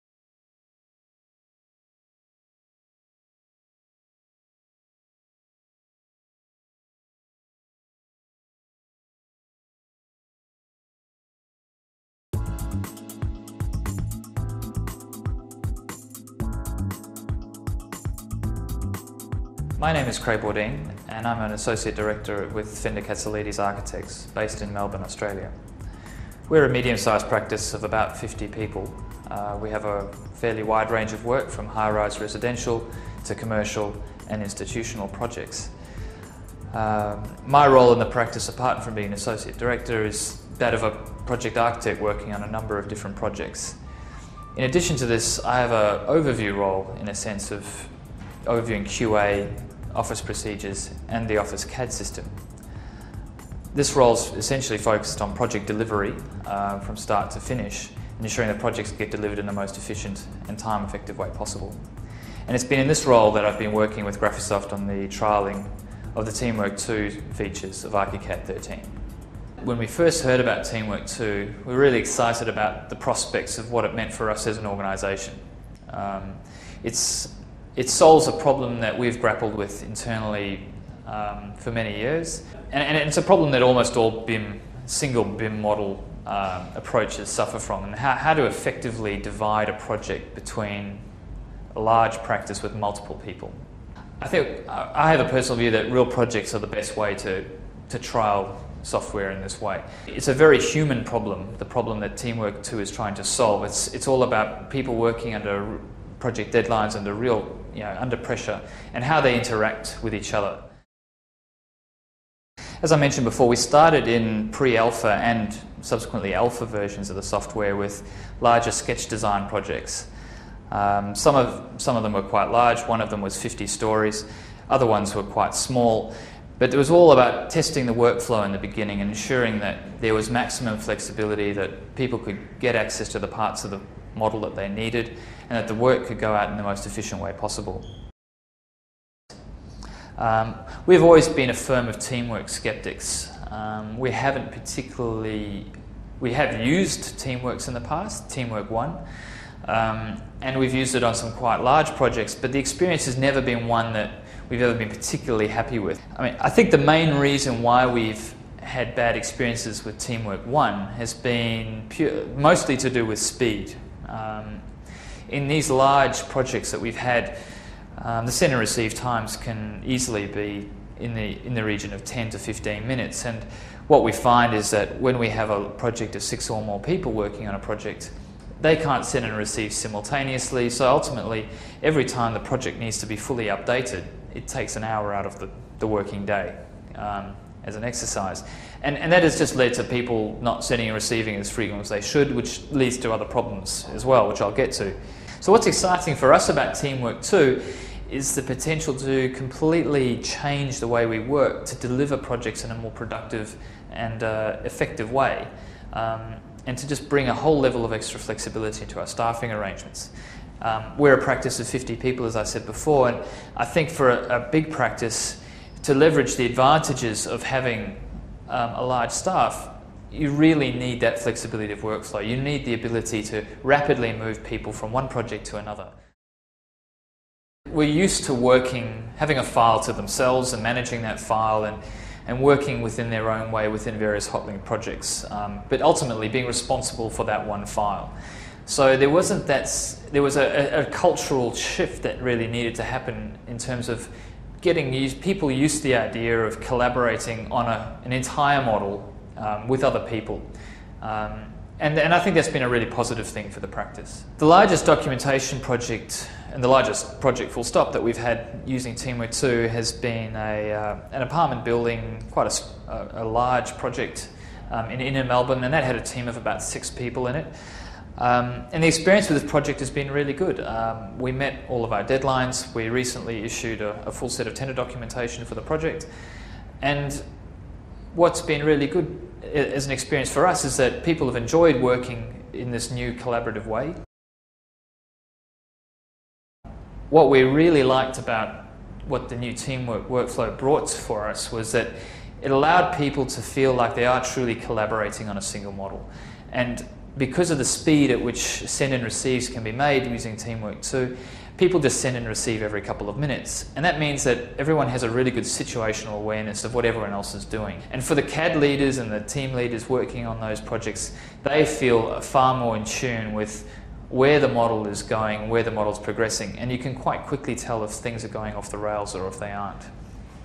Great Architects on GRAPHISOFT.com - Craig Baudin - Fender Katsalidis Architects - Part 1
Great Architects
A glance behind the scenes - their inspirations, their vision and advice to young architects.

Craig Baudin
Associate Director
Fender Katsalidis Architects
Victoria, Australia

Craig Bauding has a Bachelor's Degree in Architecture and is a registered Architect. Prior to joining FKA he worked on a number of large residential, commercial and mixed use projects in Australia, Hong Kong and Malaysia. In September 2007 he was made an Associate Director of the practice. Craig brings a broad based range of experience across all phases of the project cycle, from early concept through to completion.